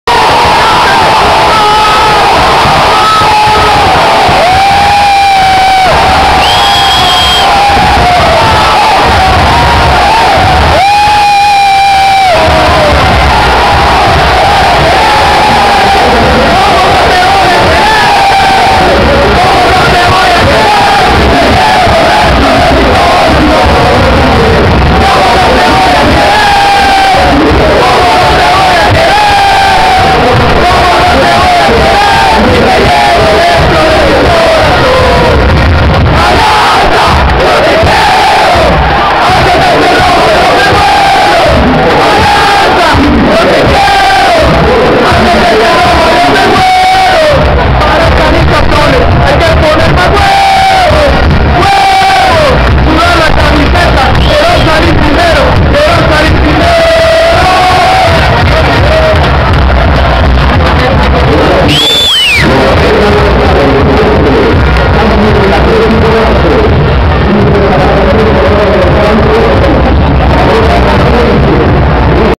Celebracion 2° Gol de Alianza F.C. Fito Zelaya 2 vs 1 marte, 16/09/2012 (El Regreso de Fito)
Rodolfo Zelaya "22" Regresa como los Grandes!!!